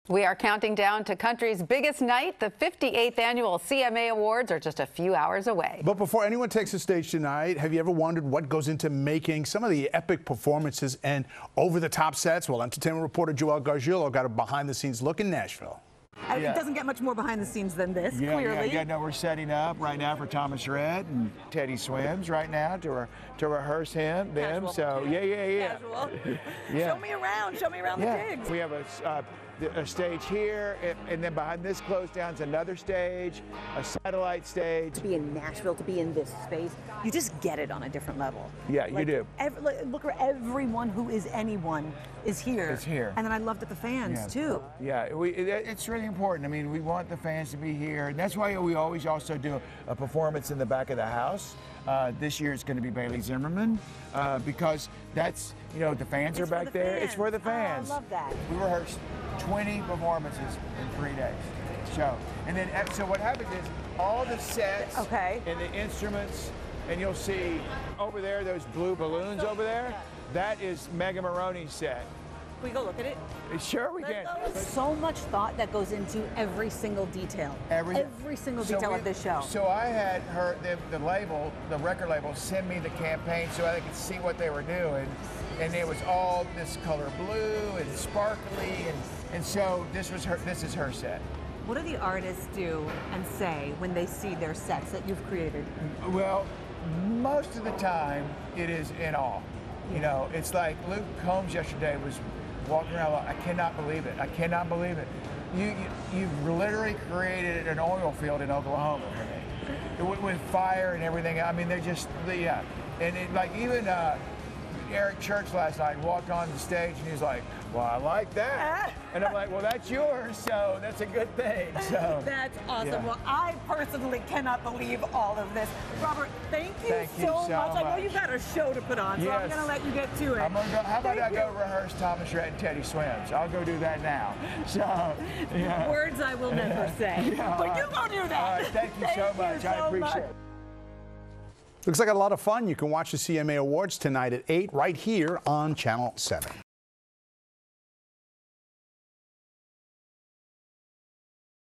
Final countdown to the CMA Awards set for tonight in Nashville, Tennessee
Entertainment Reporter Joelle Garguilo has a preview ahead of tonight's CMA Awards in Nashville, Tennessee.